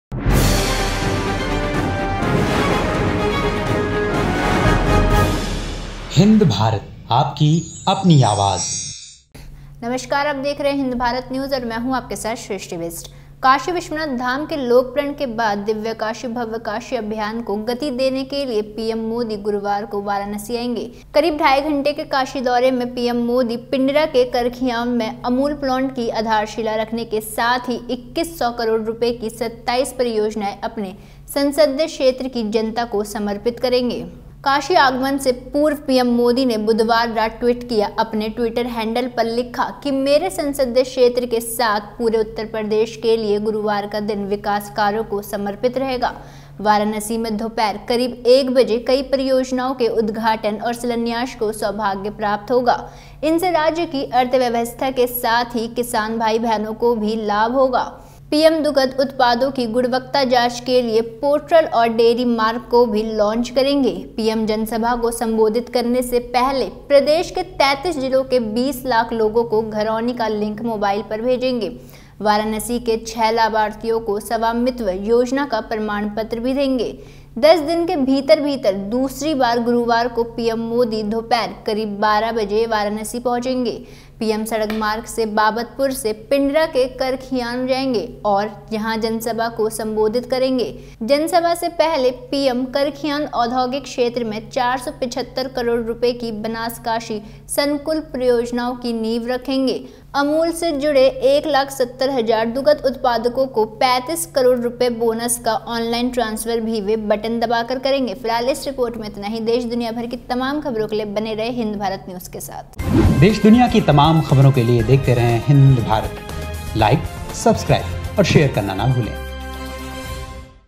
21 सौ करोड़ रुपये की 27 परियोजनाएं जनता को सौंपेंगे Pm Modi | Pm Modi Varanasi Visit | Pm Modi News
21 सौ करोड़ रुपये की 27 परियोजनाएं जनता को सौंपेंगे Pm Modi | Pm Modi Varanasi Visit | Pm Modi News